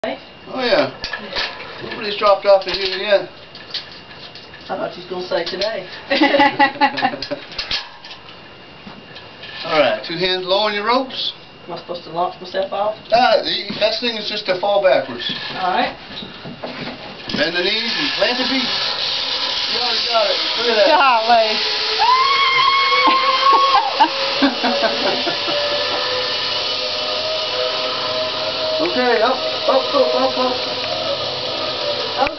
Wahoo Ziplines - Kristy
I have fun and zip upside down!